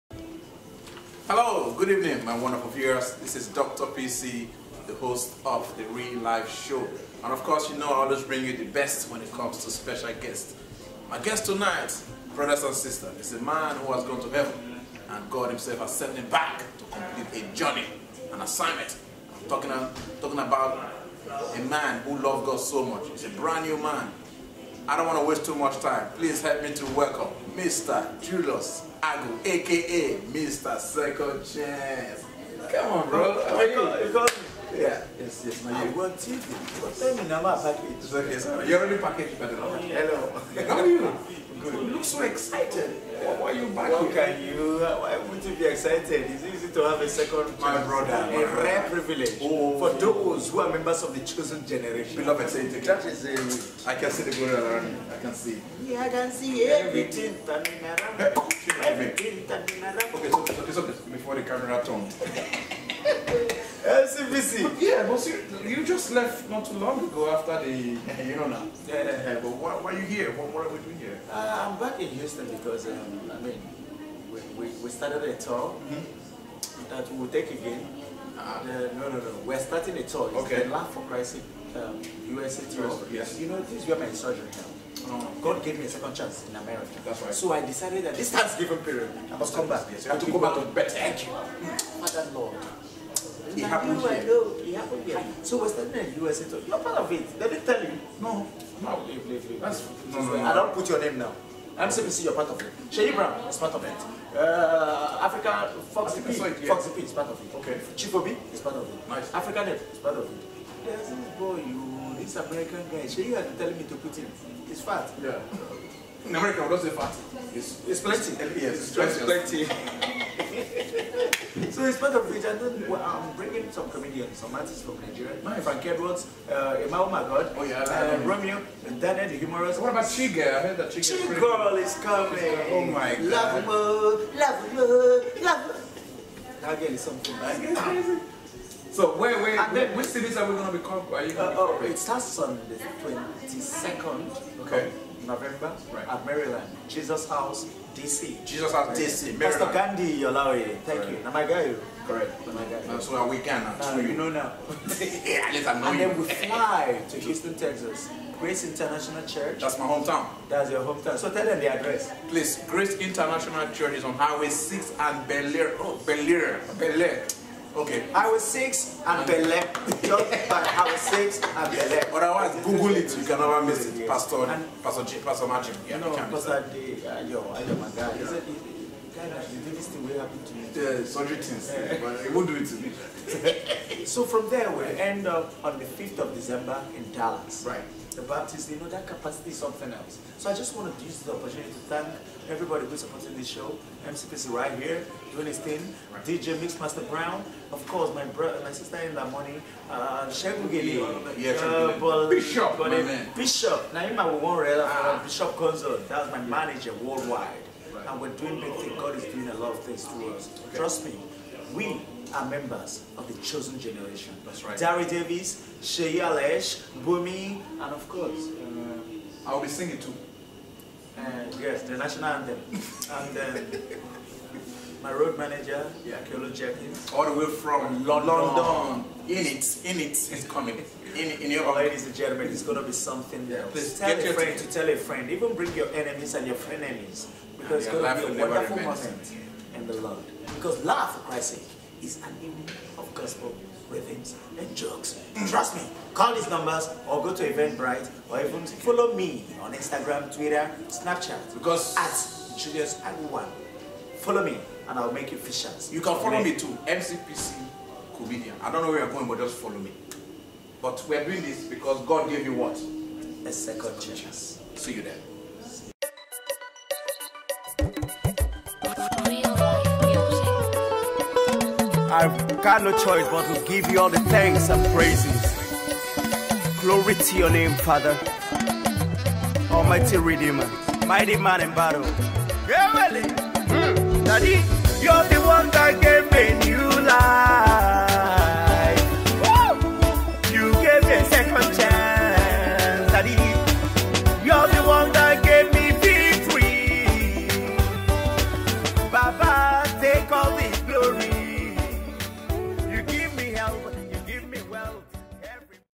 Julius Agwu interviewed by MCPC - Laff 4 Christ Sake USA Tour
LAFF 4 Christ Sake US COMEDY TOUR with JULIUSD’GENIUSAGWU and friends; FoxyP, Seyi Brown, MCPC, Aphricanape, Chief Obi, Chigul, Senator, EmmaOhMyGod, Daniel D Humorous, Romeo. Music by: Frank Edward, Seyi Alesh, Dare David, Bumie,Billy.d.Mitchell DJ Mixmaster Brown and Cooldjjamstar

Sunday, November 22nd, 2015
RCCG Jesus House DC
919/921 Philadelphia Ave,

  Tickets are available at

Event Powered by: Julius Agwu Foundation Inc (JAF), ReelLaif LLC & Bishop Entertainment Consult